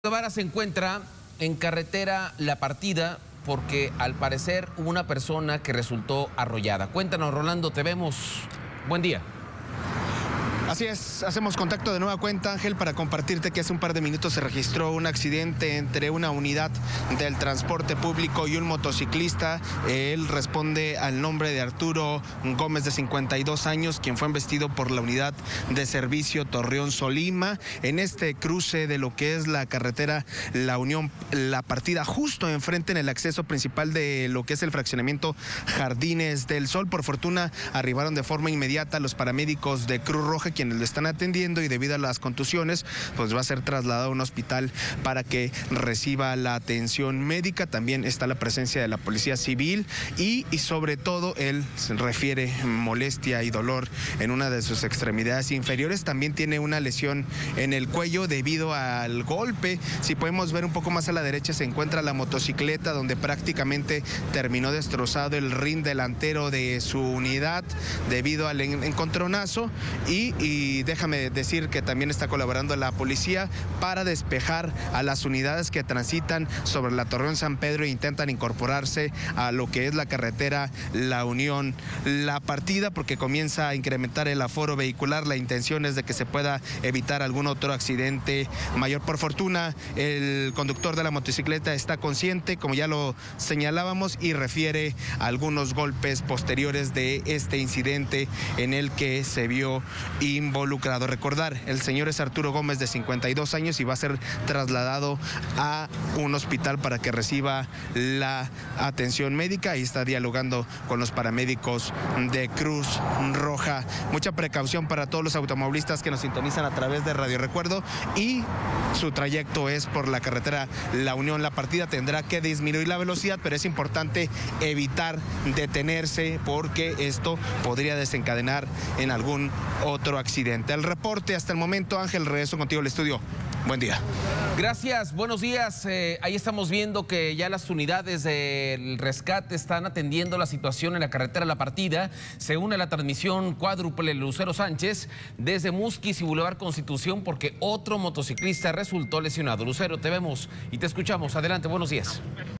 Arrollan a motociclista en vivo
En la carretera La Partida, un conductor de una motocicleta fue arrollado; de inmediato fue trasladado al hospital.